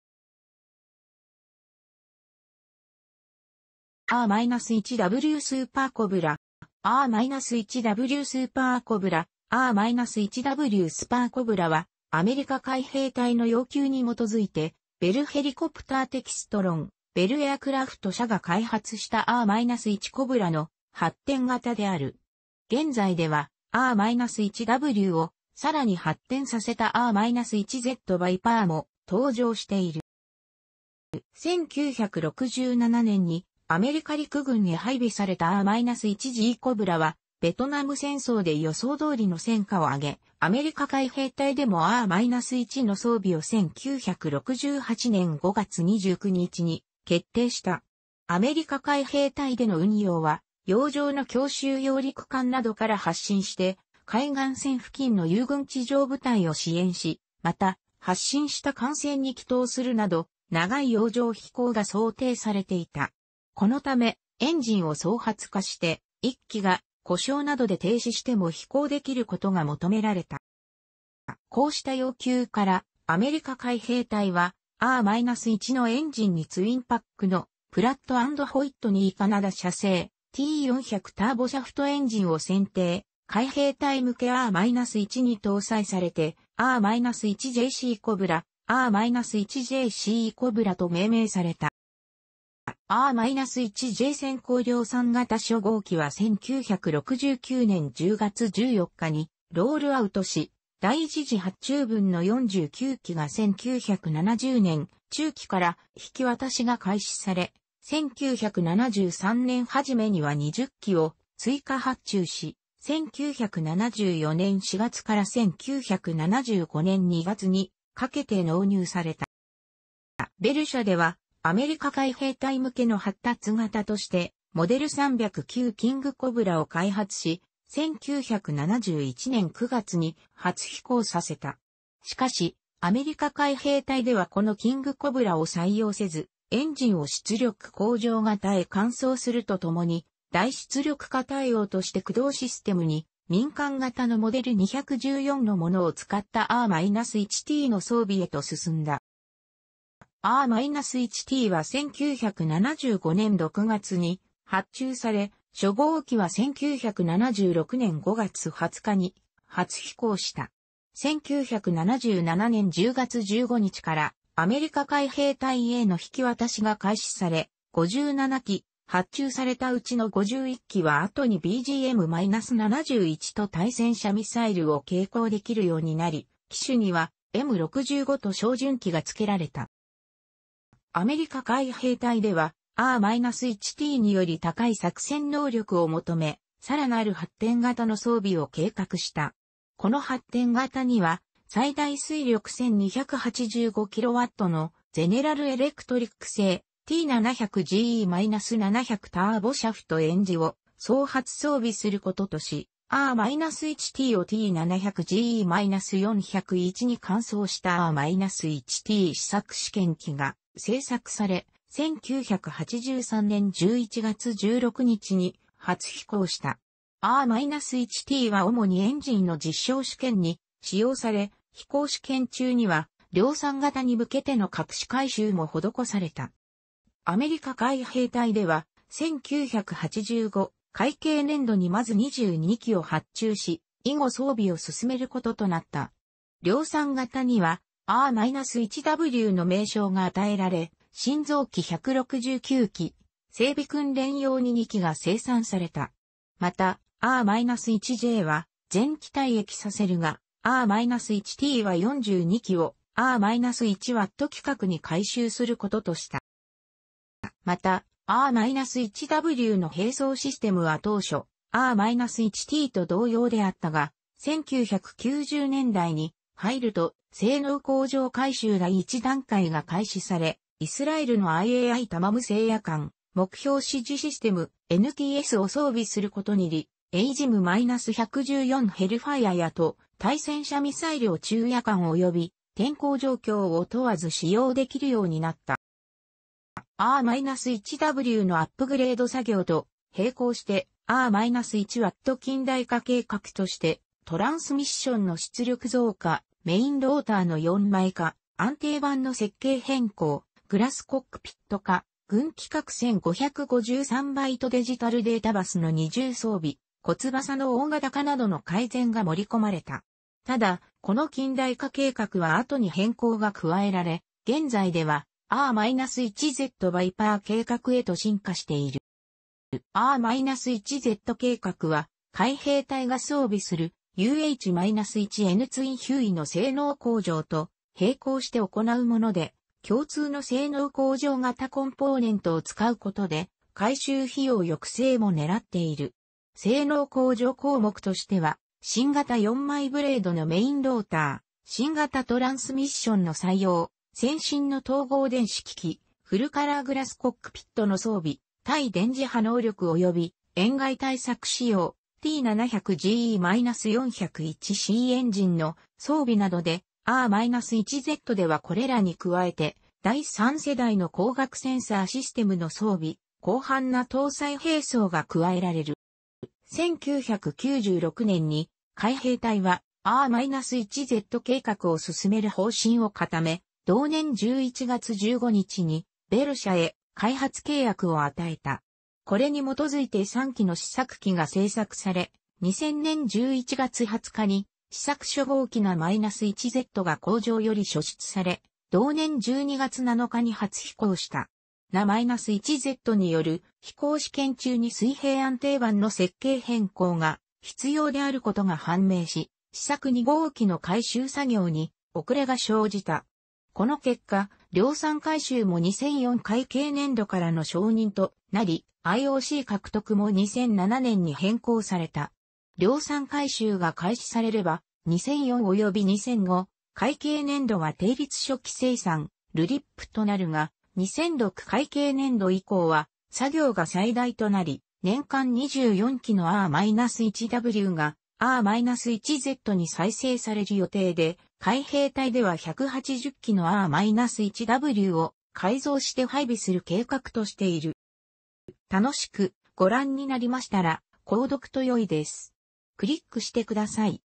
AH-1W スーパーコブラ
AH-1W スーパーコブラ（AH-1W SuperCobra）は、アメリカ海兵隊の要求に基づいてベル・ヘリコプター・テキストロン（ベル・エアクラフト）社が開発したAH-1 コブラの発展型である。現在では、AH-1Wをさらに発展させたAH-1Z ヴァイパーも登場している。

1967年にアメリカ陸軍へ配備されたAH-1G コブラは、ベトナム戦争で予想通りの戦果を上げ、アメリカ海兵隊でもAH-1の装備を1968年5月29日に決定した。アメリカ海兵隊での運用は、洋上の強襲揚陸艦などから発進して海岸線付近の友軍地上部隊を支援し、また、発進した艦船に帰投するなど、長い洋上飛行が想定されていた。このため、エンジンを双発化して、1基が故障などで停止しても飛行できることが求められた。

こうした要求からアメリカ海兵隊は、AH-1のエンジンにツインパックのプラット・アンド・ホイットニー・カナダ社製T400ターボシャフトエンジンを選定、海兵隊向けAH-1に搭載されてAH-1J シーコブラ（AH-1J SeaCobra）と命名された。

AH-1J先行量産型初号機は1969年10月14日にロールアウトし、第一次発注分の49機が1970年中期から引き渡しが開始され、1973年初めには20機を追加発注し、1974年4月-1975年2月にかけて納入された。

ベル社では、アメリカ海兵隊向けの発達型としてモデル309 キングコブラを開発し、1971年9月に初飛行させた。しかし、アメリカ海兵隊ではこのキングコブラを採用せず、エンジンを出力向上型へ換装するとともに大出力化対応として駆動システムに民間型のモデル214のものを使ったAH-1Tの装備へと進んだ。

AH-1Tは1975年6月に発注され、初号機は1976年5月20日に初飛行した。1977年10月15日からアメリカ海兵隊への引き渡しが開始され、57機発注されたうちの51機は後にBGM-71 TOW対戦車ミサイルを携行できるようになり、機首にはM65 TOW照準器が付けられた。

アメリカ海兵隊ではAH-1Tにより高い作戦能力を求め、さらなる発展型の装備を計画した。この発展型には最大推力1,285kWのゼネラル・エレクトリック製T700-GE-700ターボシャフト・エンジンを双発装備することとし、AH-1TをT700-GE-401に換装したAH-1T+試作試験機が製作され、1983年11月16日に初飛行した。AH-1T+は主にエンジンの実証試験に使用され、飛行試験中には量産型に向けての各種改修も施された。

アメリカ海兵隊では、1985会計年度にまず22機を発注し、以後装備を進めることとなった。量産型にはAH-1Wの名称が与えられ、新造機169機、整備訓練用に2機が生産された。また、AH-1Jは全機退役させるが、AH-1Tは42機をAH-1W規格に改修することとした。

また、AH-1Wの兵装システムは当初、AH-1Tと同様であったが、1990年代に入ると性能向上改修第1段階が開始され、イスラエルのIAI-タマム製夜間目標指示システム（NTS）を装備することにより、AGM-114ヘルファイアやTOW対戦車ミサイルを昼夜間および天候状況を問わず使用できるようになった。

AH-1Wのアップグレード作業と並行して、AH-1W近代化計画として、トランスミッションの出力増加、メインローターの4枚化、安定板の設計変更、グラスコックピット化、軍規格1553Bデジタル・データバスの二重装備、小翼の大型化などの改善が盛り込まれた。ただ、この近代化計画は後に変更が加えられ、現在ではAH-1Z ヴァイパー計画へと進化している。

AH-1Z計画は、海兵隊が装備するUH-1N ツインヒューイの性能向上と並行して行うもので、共通の性能向上型コンポーネントを使うことで改修費用抑制も狙っている。性能向上項目としては、新型4枚ブレードのメインローター、新型トランスミッションの採用、先進の統合電子機器/フルカラー・グラスコックピットの装備、対電磁波能力および塩害対策仕様、T700-GE-401Cエンジンの装備などで、AH-1Zではこれらに加えて、第3世代の光学センサー・システムの装備、広範な搭載兵装が加えられる。

1996年に海兵隊はAH-1Z計画を進める方針を固め、同年...